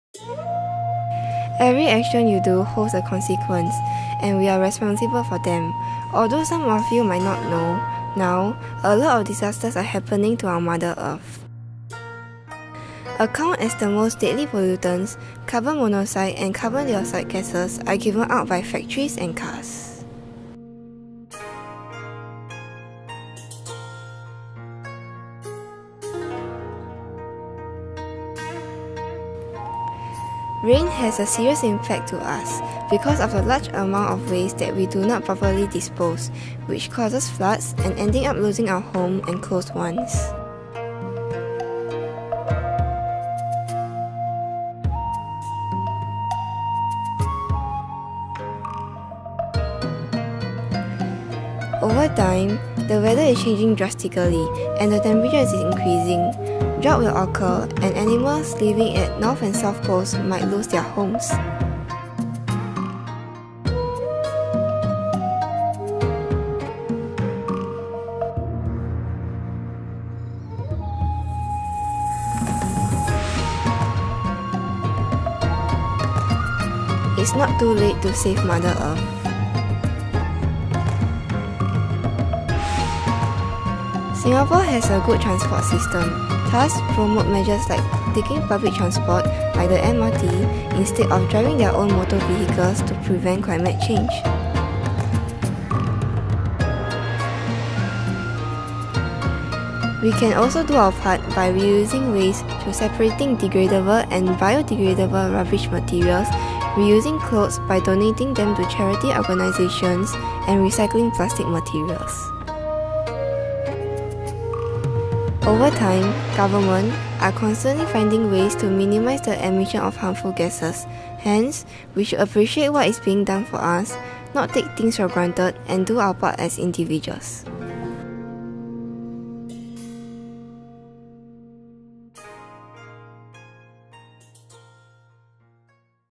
NCCC2012 Short Film Challenge Vid3
A Production of Compassvale Secondary School Infocomm Club. An entry for the National Climate Change Competition (NCCC) 2012 Short Film Challenge. Filmed and edited by Agnes Ho Sin Xuan, Tan Yun Ru, Teo Hui Yi, Natasya Nathra.